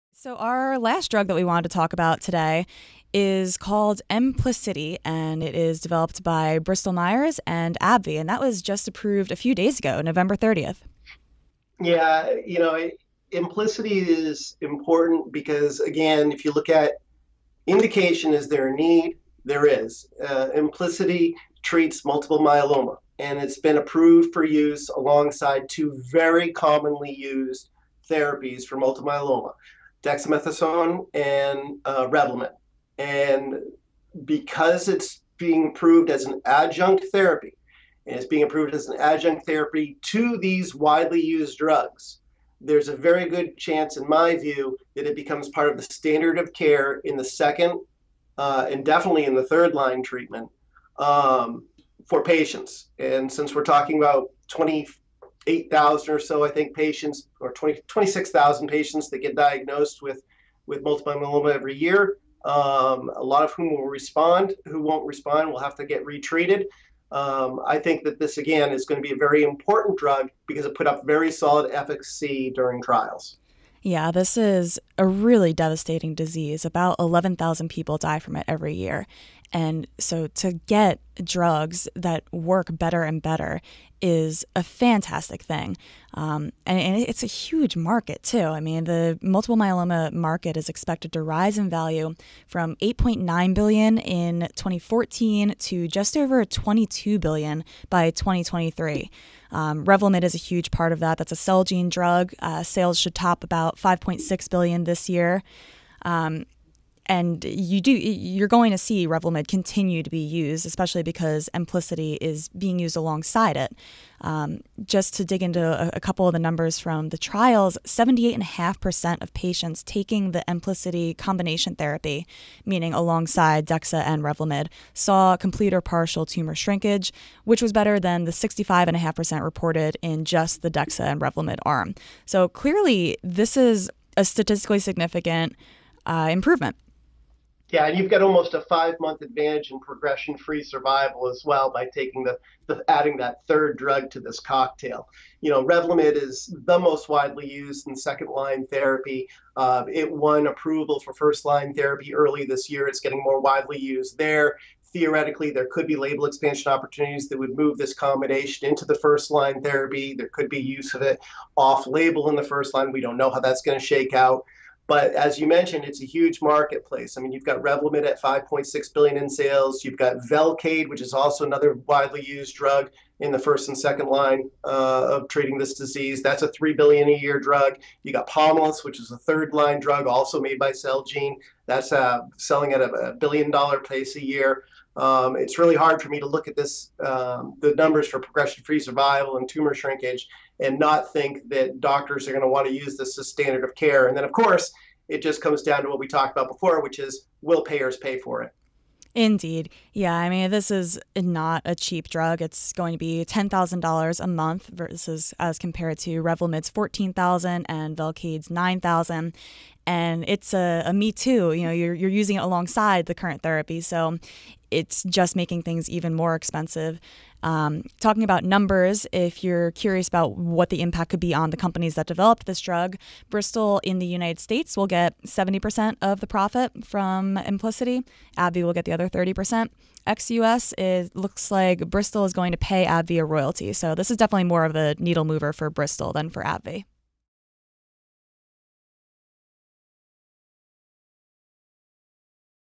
Top Drug Approvals of 2015: Teaming Up in Multiple Myeloma
Doctors get a new weapon in the battle back against multiple myeloma.

This podcast was recorded on 12/2/2015.